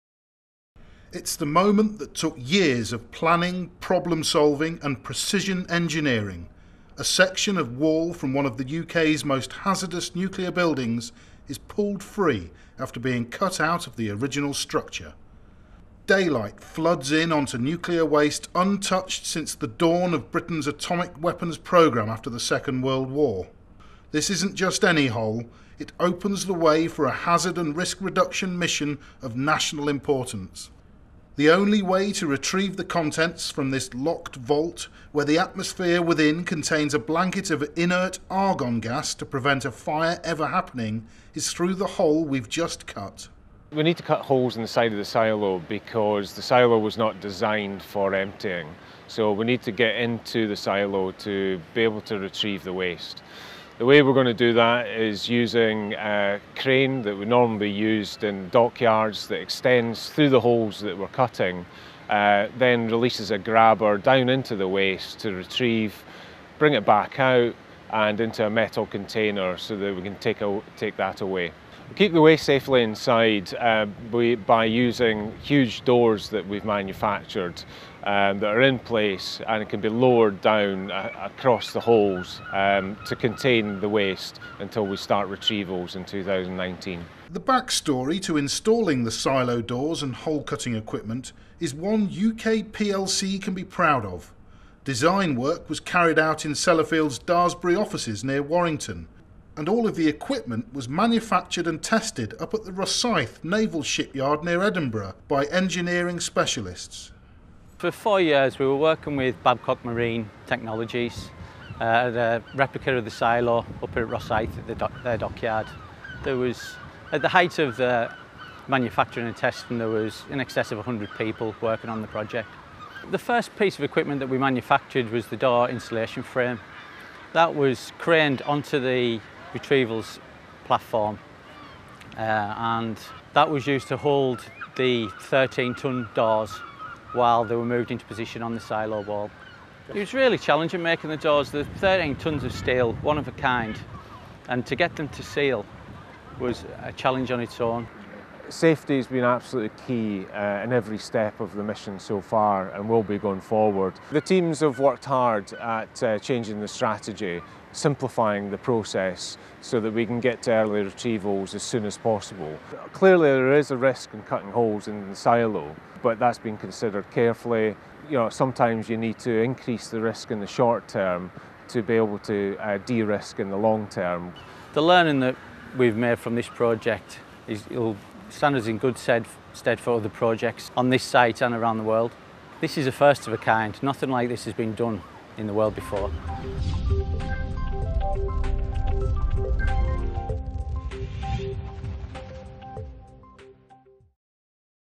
The most complicated ‘hole in a wall’ ever made?
How Sellafield Ltd opened up its radioactive ‘locked vault’ called the Pile Fuel Cladding Silo.